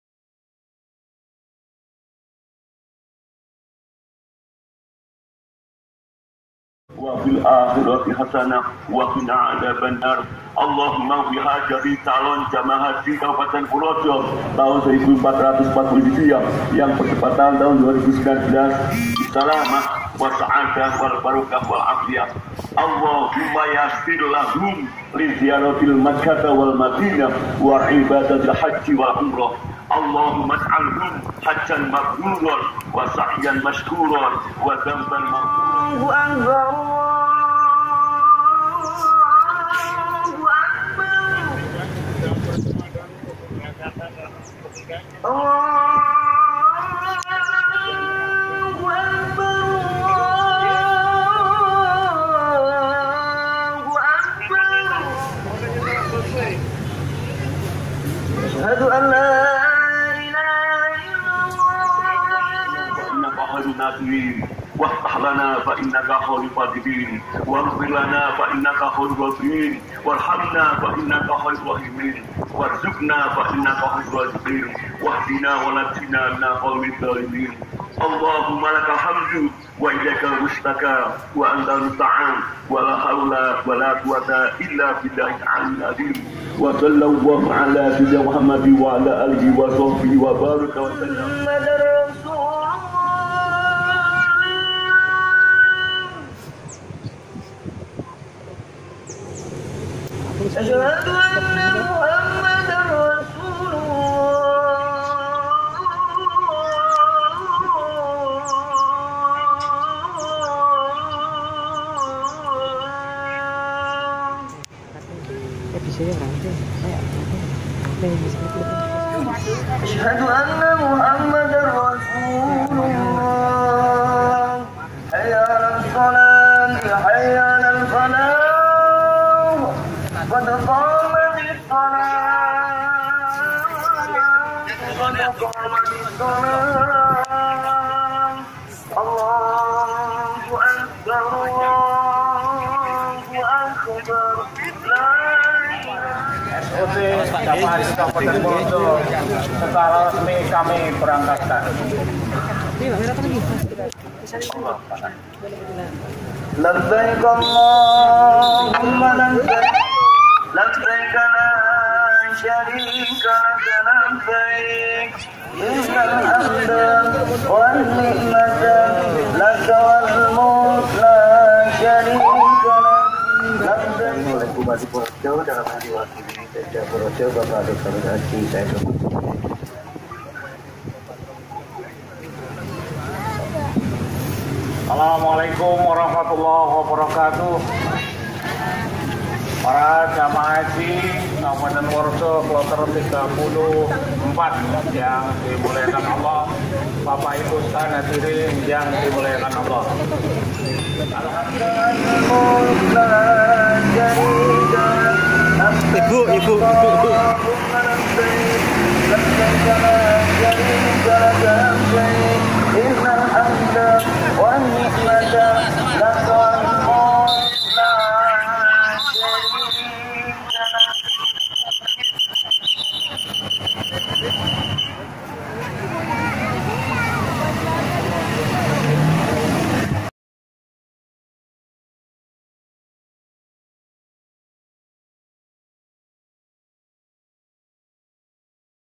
Full pelepasan jamaah haji di Kabupaten purworejo 2019 kloter terakhir
Cara Kabupaten purworejo melepaskan  para jamaah haji, semoga bermanfaat